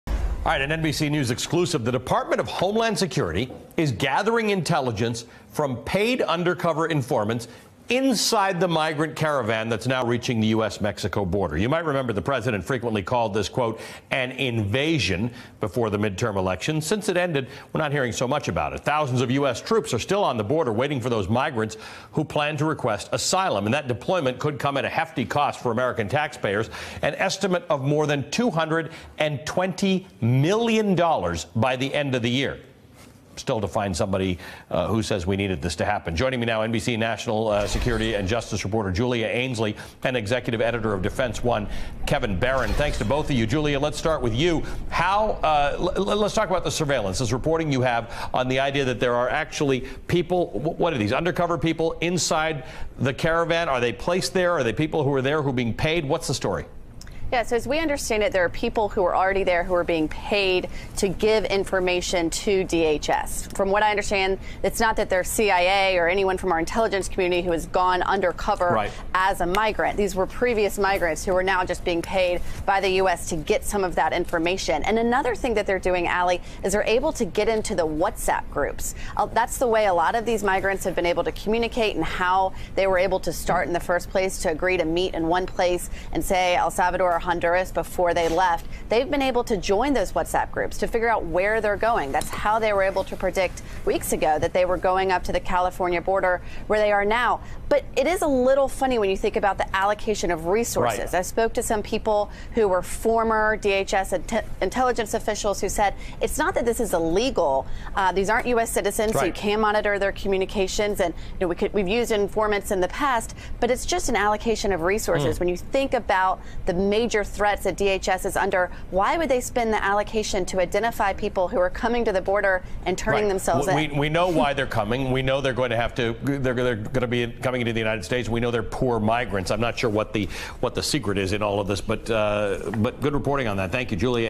Federal Agents Embedded In Caravan
The Department of Homeland Security is reportedly gathering intelligence from paid undercover informants in the migrant caravan.

According to NBC News, the 4,000 migrants have been using WhatsApp to communicate during their journey to the US-Mexico border.

DHS personnel joined those WhatsApp groups to monitor the messages of migrants.

Nearly 3,000 people from the first of the caravans arrived in Tijuana, Mexico, on Monday.

DHS officials reportedly told NBC that they are also receiving reports from DHS personnel working in Mexico with the government there in an effort to keep tabs on the caravan's size, movements and any potential security threats.

On Monday, DHS officials told reporters that their informants on Sunday had indicated that a group of migrants wanted to run through the car lanes of a border crossing near San Diego.

As a result, the US closed off northbound traffic for several hours at the busiest border entry point from Mexico to install new security barriers 3am to 6am.

Customs and Border Protection agents also installed movable, wire-topped barriers. But nothing was ever attempted.

The news comes after it was reported that more than 500 criminals were found traveling in the caravan.

Officials would not reveal how they identified the criminals, but said most of the individuals were adult men and teenage boys, according to Fox News.

On Monday, a federal judge barred the Trump administration from refusing asylum to immigrants who cross the southern border illegally.

President Donald Trump issued a proclamation on November 9 that said anyone who crossed the southern border between official ports of entry would be ineligible for asylum.

Trump said an asylum ban was necessary to stop what he's attacked as a national security threat.

But in his ruling Monday, US District Judge Jon Tigar agreed with legal groups that immediately sued, arguing that US immigration law clearly allows someone to seek asylum even if they enter the country between official ports of entry.

'Whatever the scope of the President's authority, he may not rewrite the immigration laws to impose a condition that Congress has expressly forbidden,' said Tigar, a nominee of former President Barack Obama.

The Department of Homeland Security did not immediately comment on the ruling, which will remain in effect for one month barring an appeal.

In issuing the ban, Trump used the same powers he used last year to impose a travel ban that was ultimately upheld by the Supreme Court.

If enforced, the ban would potentially make it harder for thousands of people to avoid deportation. DHS estimates around 70,000 people a year claim asylum between official ports of entry.

But Tigar's ruling notes that federal law says someone may seek asylum if they have arrived in the United States, 'whether or not at a designated port of arrival.'

'Individuals are entitled to asylum if they cross between ports of entry,' said Baher Azmy, a lawyer for the Center for Constitutional Rights, which sued the government alongside the American Civil Liberties Union. 'It couldn't be clearer.'

As of Monday, 107 people detained between official crossings have sought asylum since Trump's order went into effect, according to DHS, which oversees Customs and Border Protection.

DHS has said it wants asylum seekers at the southern border to appear at an official border crossing. But many border crossings such as San Ysidro already have long wait times.

People are often forced to wait in shelters or outdoor camps on the Mexican side, sometimes for weeks.

ACLU lawyer Lee Gelernt said that some people seeking asylum cross between official ports because 'they're in real danger,' either in their countries of origin or in Mexico.

'We don't condone people entering between ports of entry, but Congress has made the decision that if they do, they still need to be allowed to apply for asylum,' he said.